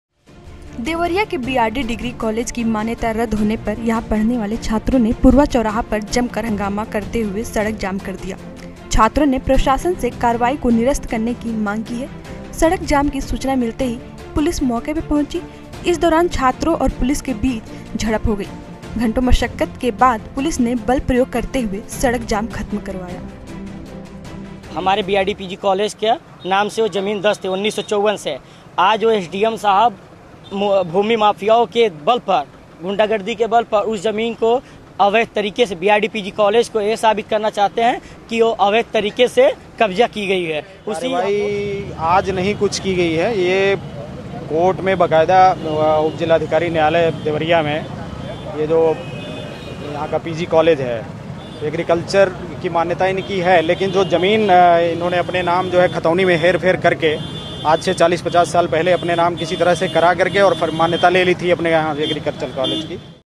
देवरिया : बीआरडी डिग्री कॉलेज की मान्यता रद, सड़क पर उतरे छात्र
देवरिया: बीआरडी डिग्री कॉलेज की मान्यता रद होने पर यहां पढ़ने वाले छात्रों ने पुरवा चौराहे पर हंगामा करते हुए सड़क पर जाम लगा दिया. छात्रों ने प्रशासन से कार्रवाई को निरस्त करने की मांग की है. जाम की सूचना मिलते ही पुलिस मौके पर पहुंची. इस दौरान छात्रों और पुलिस के बीच झड़प हो गई. घंटों मशक्कत के बाद पुलिस ने बल प्रयोग करते हुए जाम खत्म करवाया.